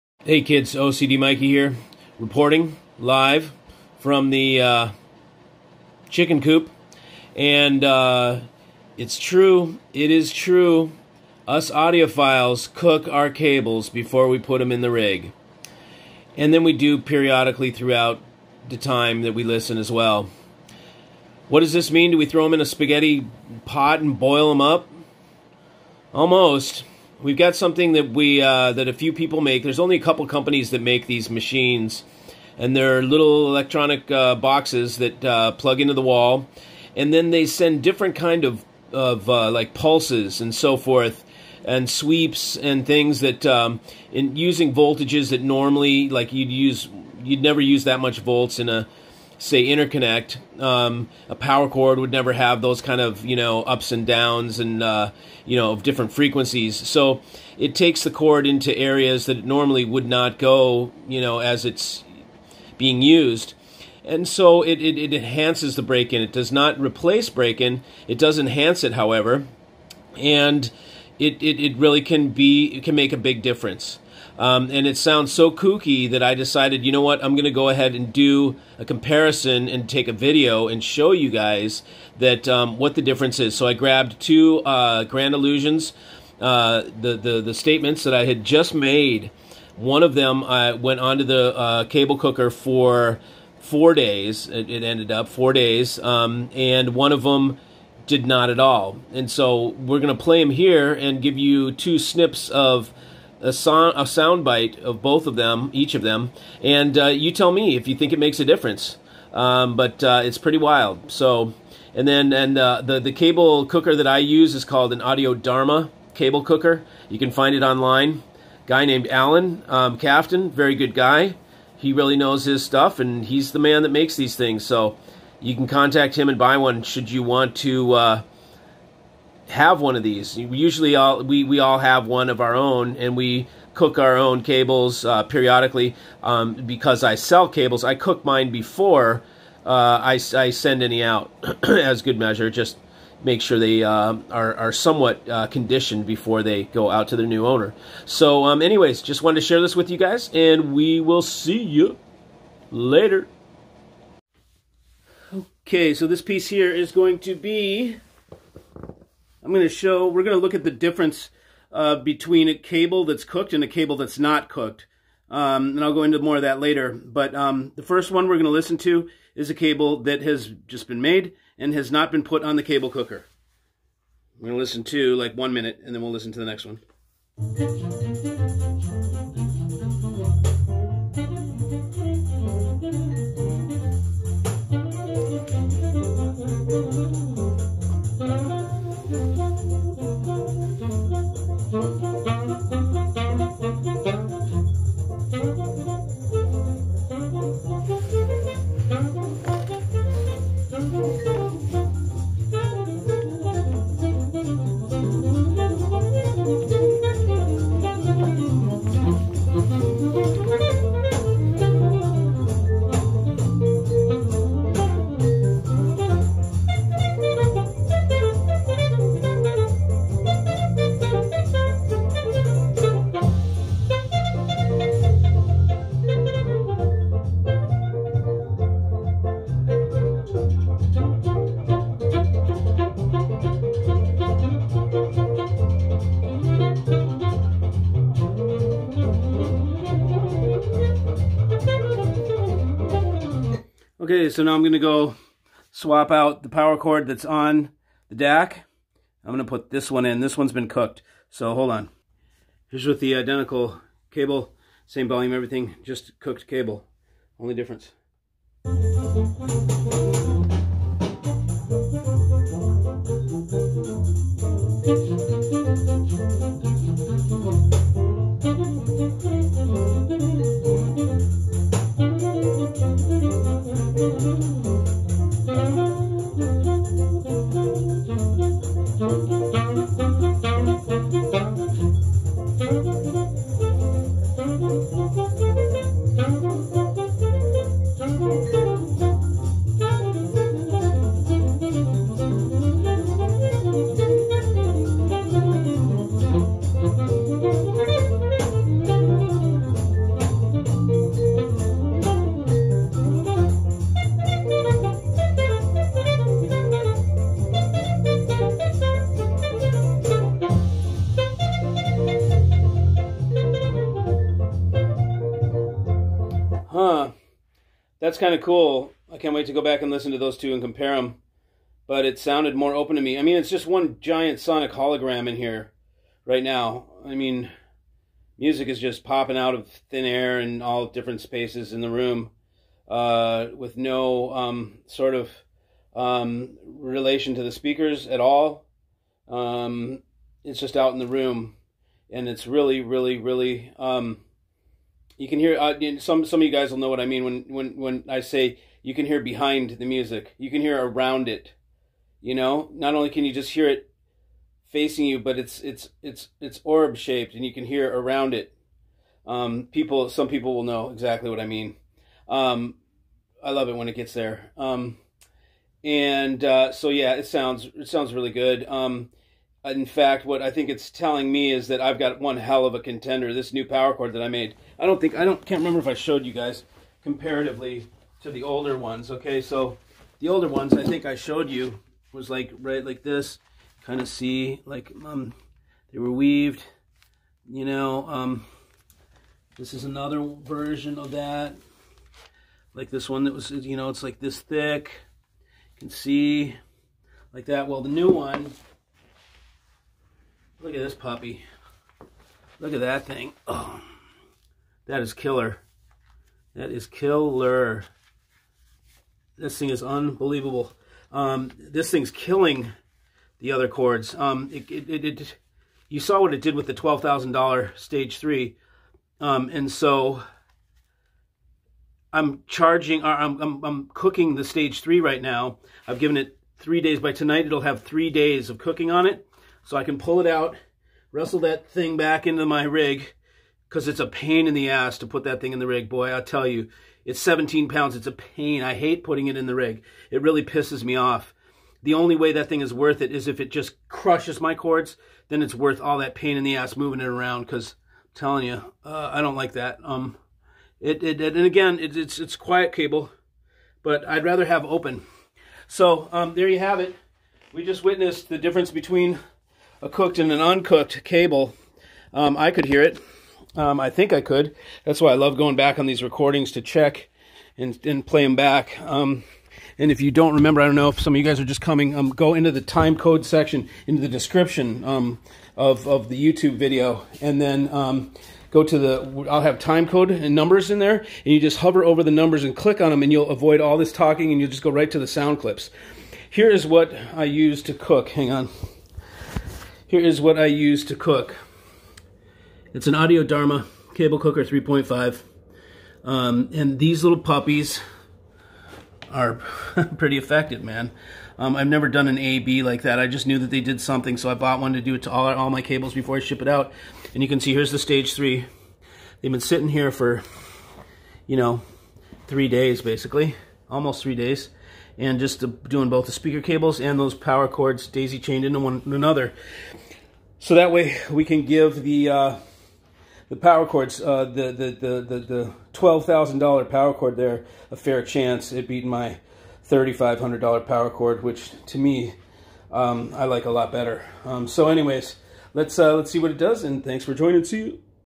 What is HiFi Cable COOKING ?!!
Thats right, us nutty Audiophiles "Cook" our cables on special machines designed to sonically condition our cables and "precharge" them up. Come take a look !